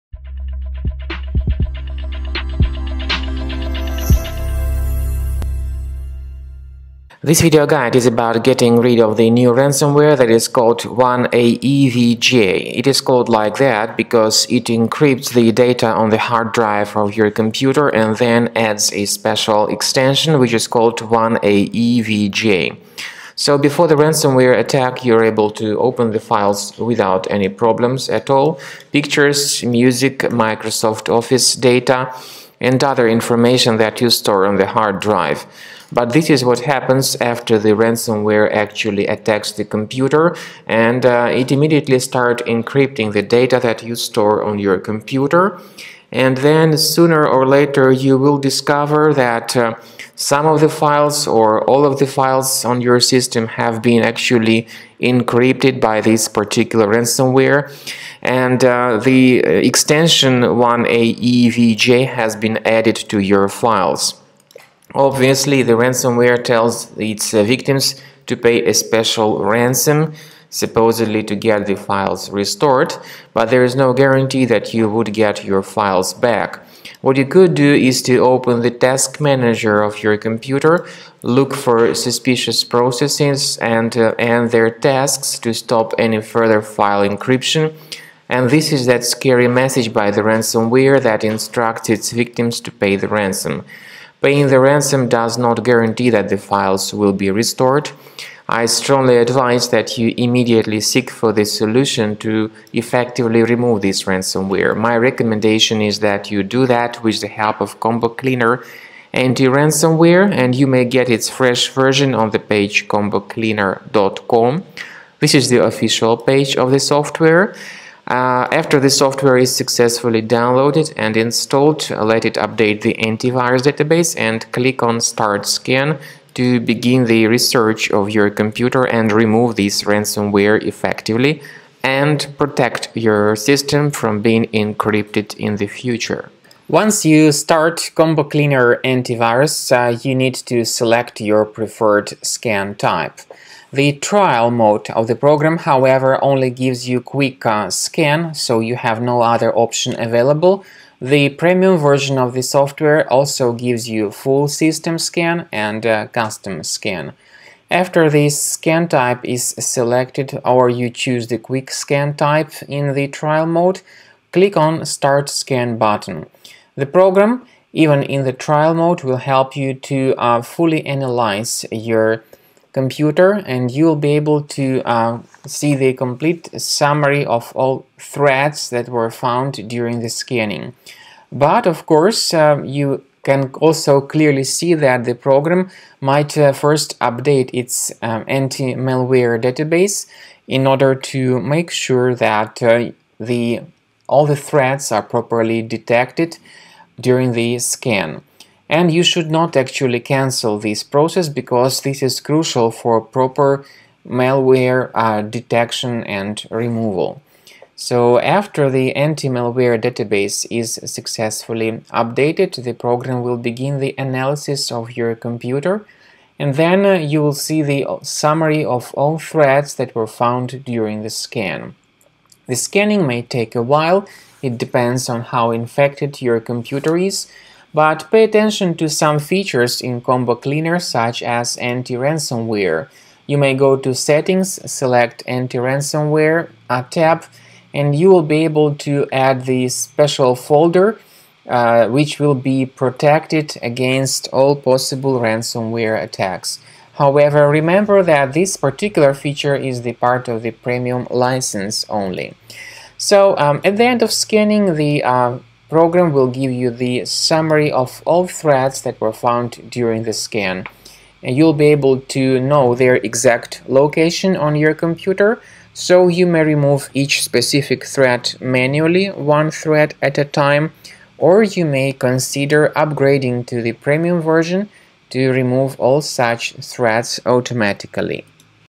1aevj ransomware. How to decrypt .1aevj files?
1aevj is a new ransomware-type infection. Currently it targets many Windows devices. It tells its victims to pay the ransom for file decryption. However, there is no guarantee that you will get your files back even if you pay the ransom. Remove it with the help of Combo Cleaner Antivirus and get your files protected with its help in the future.